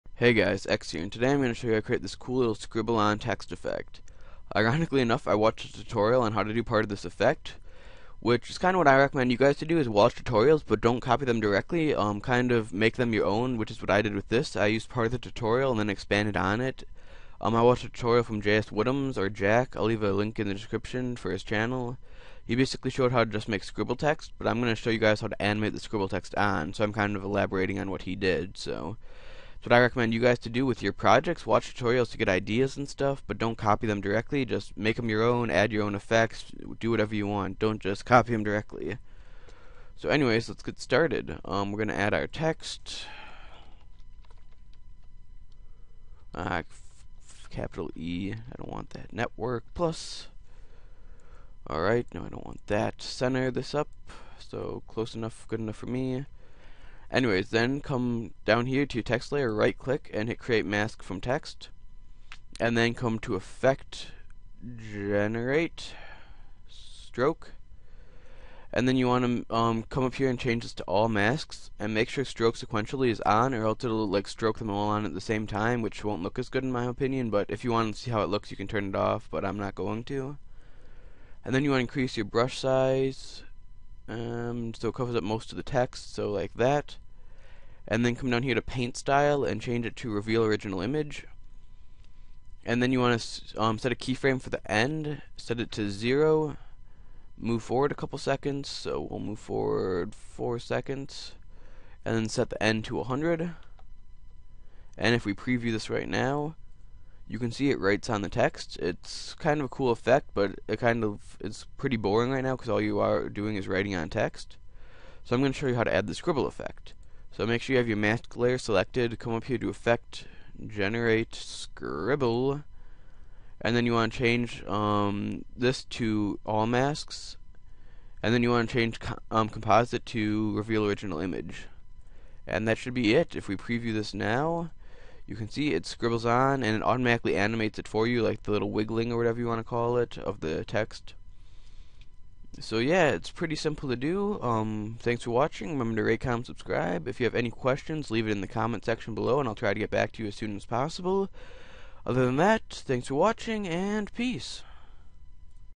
After Effects CS5 - How to Make Scribble Text
This is the way to make scribble text in After Effects.